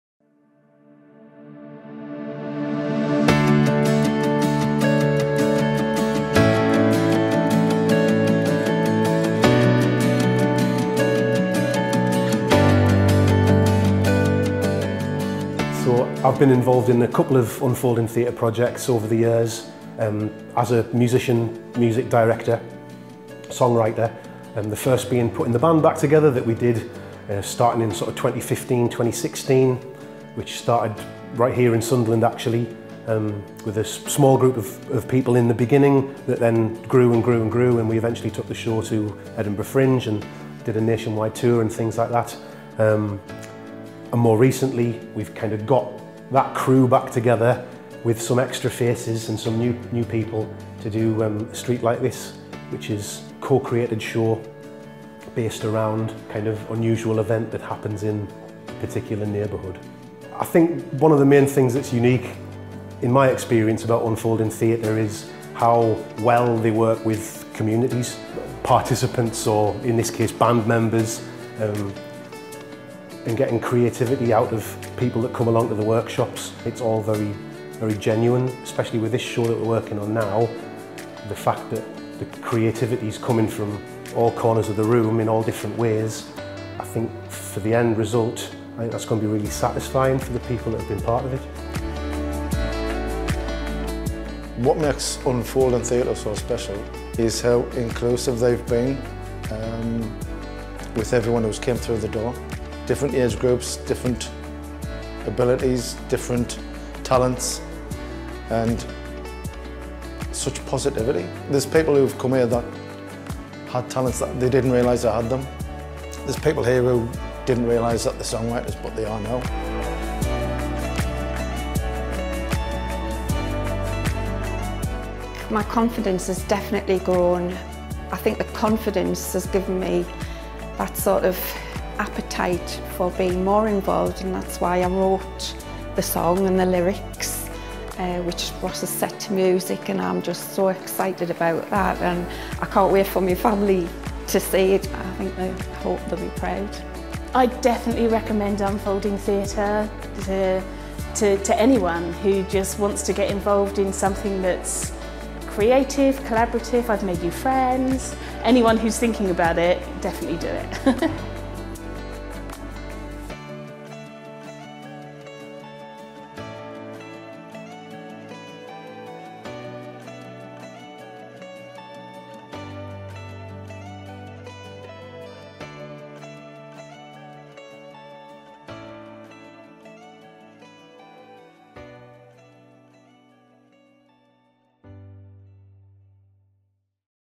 A Street Like This: a new co-created show from Unfolding Theatre
Writtenby Alison Carr (Finalist: Women’s Prize for Playwriting 2022)
Songs by Ross Millard (The Futureheads) co-created with a community company.

A Street Like This is a new production from Unfolding Theatre about the unexpected friendships that arise when something unforeseen happens, changing the way people define themselves forever.

The show takes us from the everyday to the extraordinary, in a music-filled theatre production that reminds us that even at a time of social division, we have more in common than keeps us apart.

Featuring original songs, co-written by Ross Millard (The Futureheads) and the company, A Street Like This is an optimistic, fantastical and often funny insight into an ordinary life lived extraordinarily.

It has been developed with a community cast in Sunderland. Working with three professional performers, the community company are the Greek chorus of our story. They sing, play music and introduce us to a very ‘normal’ street. However, when a giant sinkhole opens up in the middle of the road, the residents are thrown into a world of new possibilities.